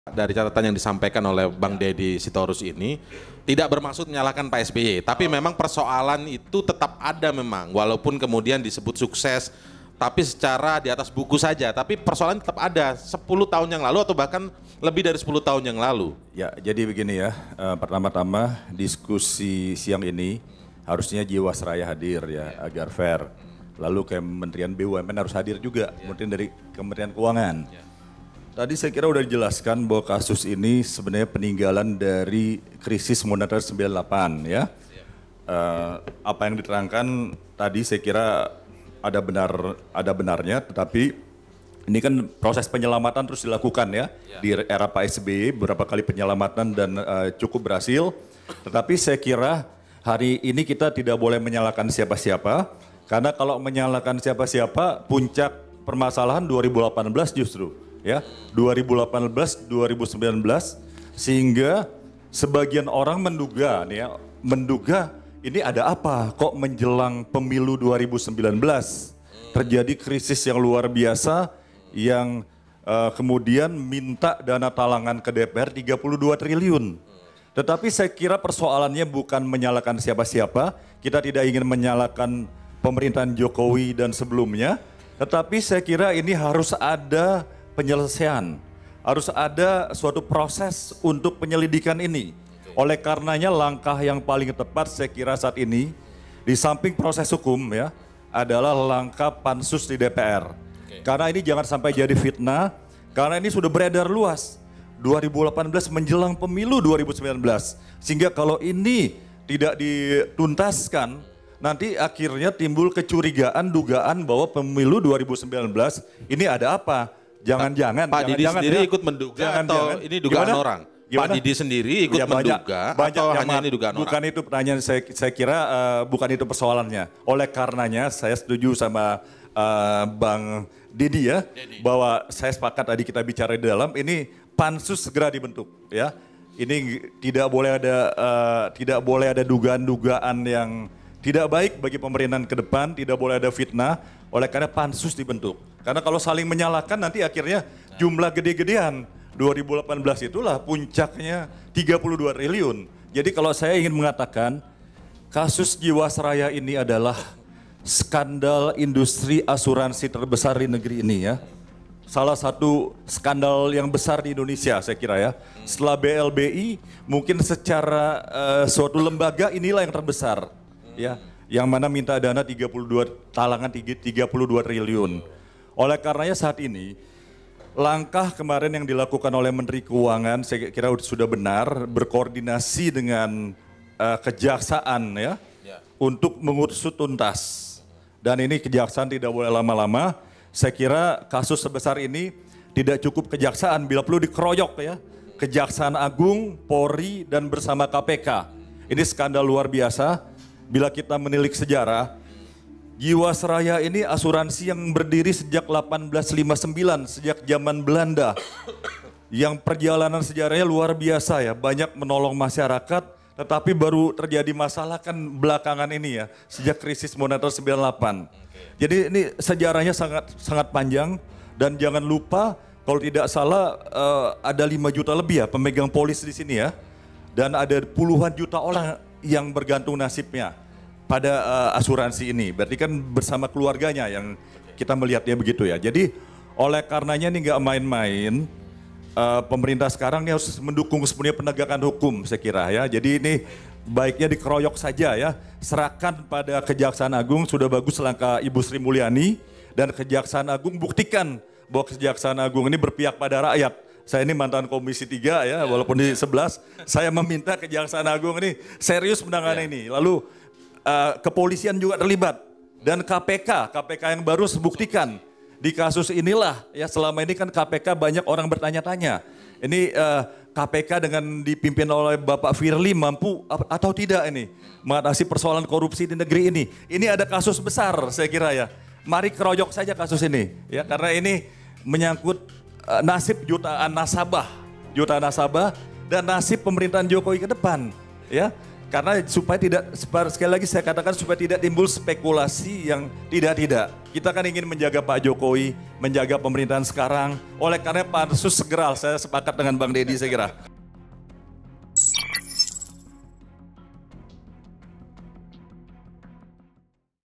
Politisi Demokrat: Jiwasraya Skandal Luar Biasa, Kok Jelang Pilpres 2019?
Wakil Sekjen Partai Demokrat Didi Irawadi Syamsudin menilai bahwa kasus PT Asuransi Jiwasraya merupakan skandal terbesar industri asuransi di Indonesia. Bahkan, Diri Irawadi menilai bahwa kasus Jiwasraya jadi skandal terbesar setelah skandal Bantuan Likuiditas Bank Indonesia (BLBI). Pasalnya, dalam kasus ini Jiwasraya meminta dana talangan yang jumlahnya mencapai Rp 32 triliun.